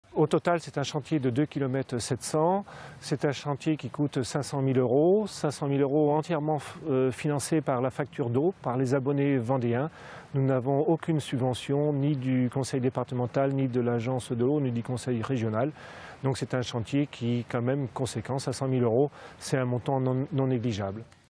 Travaux d'eau en Vendée : Interview d’Eric Rambaud
Sites webs/blogs, copiez cette vidéo avec le bouton "Intégrer" !
 Interview d’Eric Rambaud, président de Vendée Eau sur d’importants travaux menés sur les réseaux d’eau portable du département entre le Château d'eau du Fuiteau et l'usine de l'Angle Guignard.